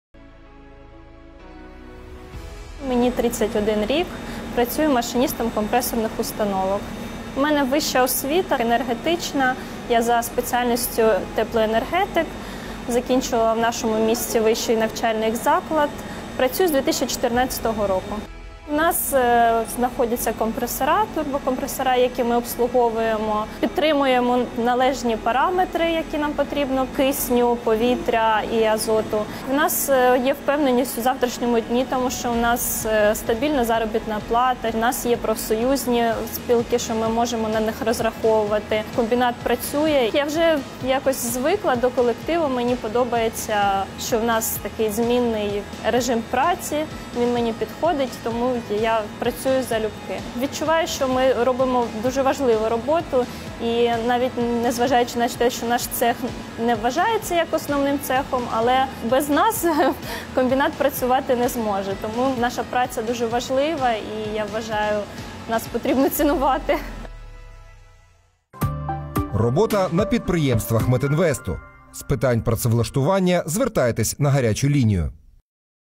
Машиніст компресорних установок Каметсталі Анастасія Павленко
Молода жінка – фахівчиня-теплоенергетик із вищою освітою, яку отримала у рідному місті за свідомим професійним вибором, адже вважає сучасну енергетику справою цікавою і перспективною.